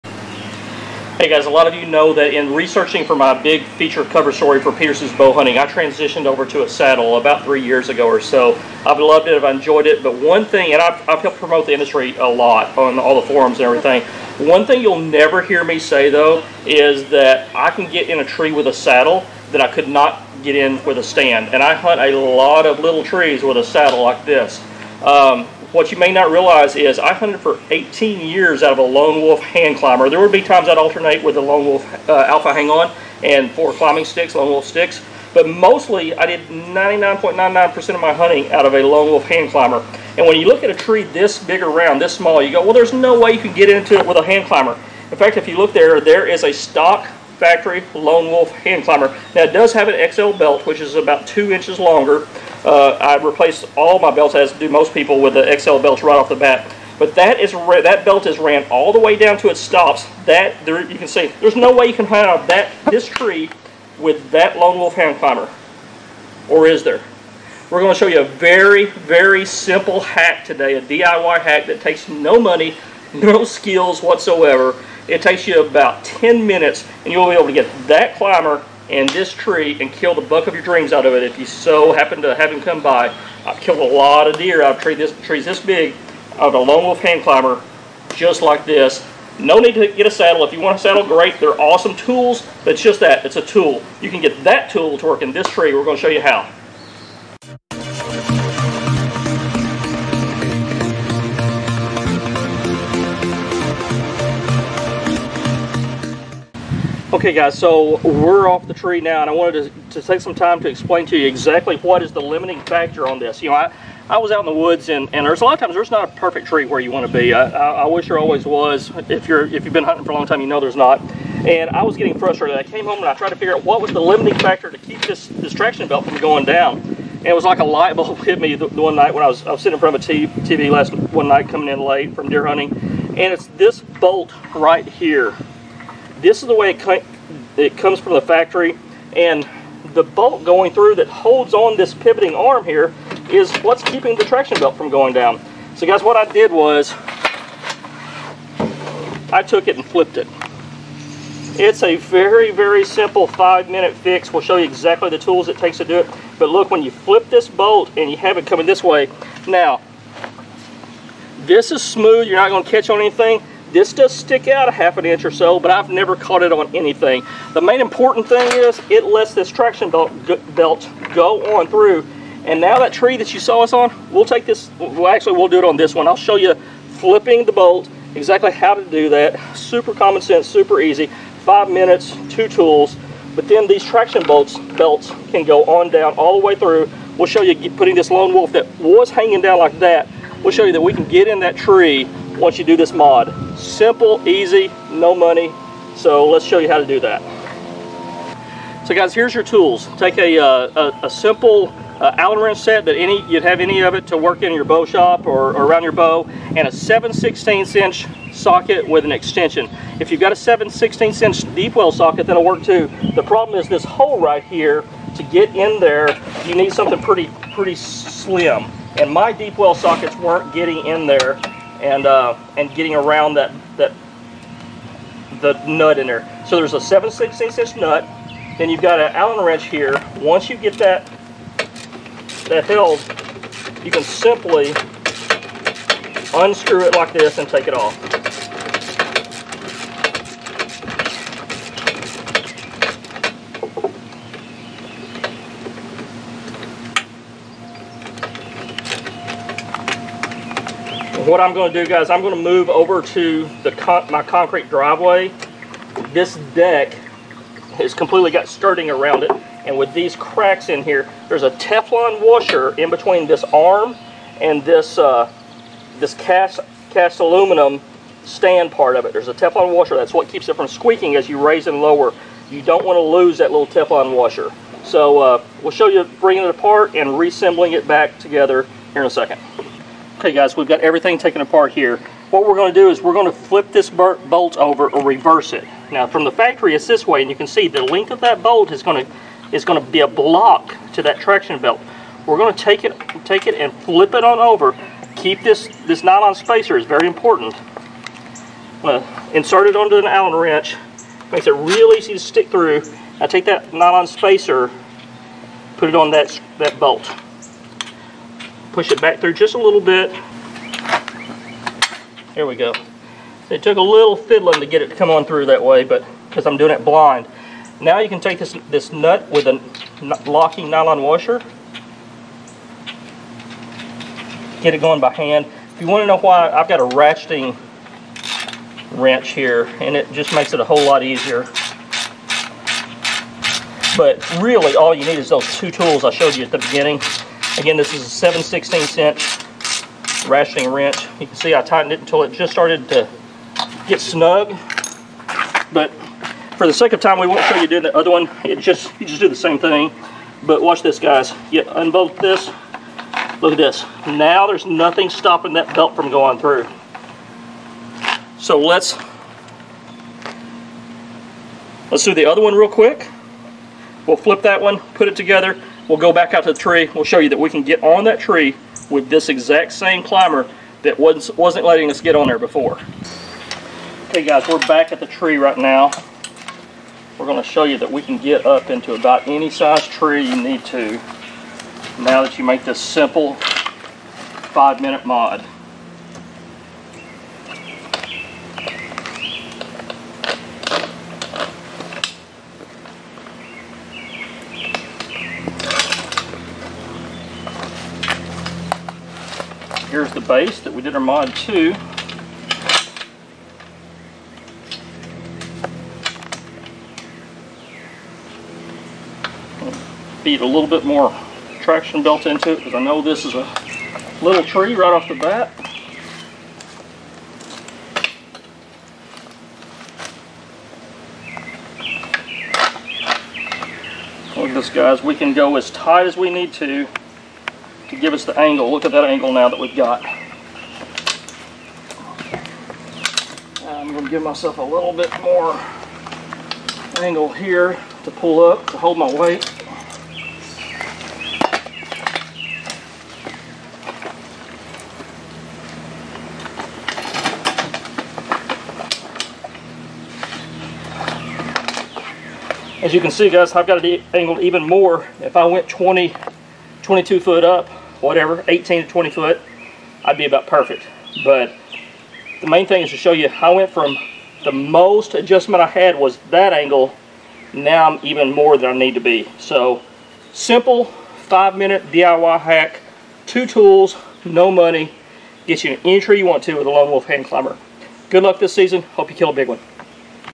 Lone Wolf Hand Climber DIY Hack: Get In Any Size Tree In 5 Minutes
Ever wanted to climb a tree that was in the perfect spot with your LW hand climber but that tree was too small? Never have that challenge again after watching this video!